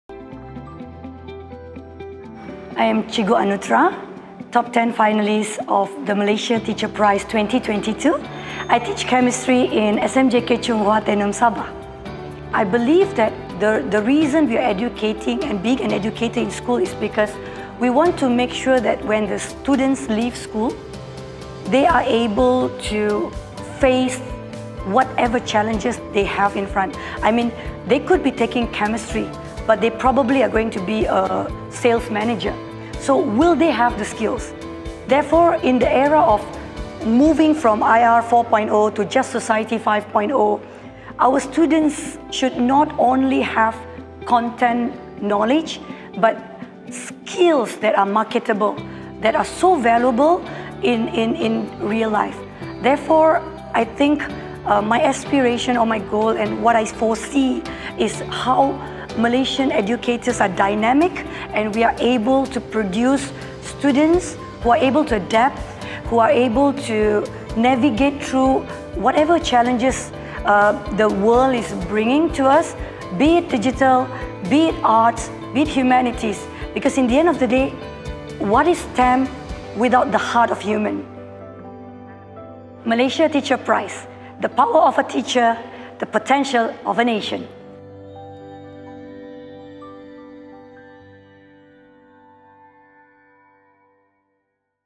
Cikgu Anuthra (Winner of Malaysia Teacher Prize 2022)
Cikgu Anuthra ialah pemenang Malaysia Teacher Prize 2022. Beliau merupakan guru bagi subjek Kimia dari SMJK Chung Hwa Tenom, Sabah.

Kejayaan anak murid yang bernama Brenda telah menjadi pemangkin semangat beliau untuk meneruskan setiap usaha yang dijalankan untuk komuniti kawasan Tenom. Antara inovasi pengajaran yang telah dijalankan adalah mengendalikan pengajarannya menggunakan kaedah Gamifikasi, dan impak dari kaedah ini berjaya dilihat apabila subjek Kimia SPM telah menjadi antara 10 subjek yang meraih peratus keputusan yang cemerlang di Sabah.

Kepercayaan yang tinggi terhadap anak murid dan memberi kebebasan untuk mereka membuat pilihan telah menjadi prinsip asas Cikgu Anuthra. Beliau percaya setiap anak murid ini mempunyai potensi untuk berinovasi. Kepercayaan yang tinggi ini juga telah membuahkan hasil apabila anak muridnya telah memenangi beberapa pertandingan inovasi. Sejak dari itu, beliau mula merancang untuk mengembangkan dan mengekalkan setiap perubahan yang telah dijalankan.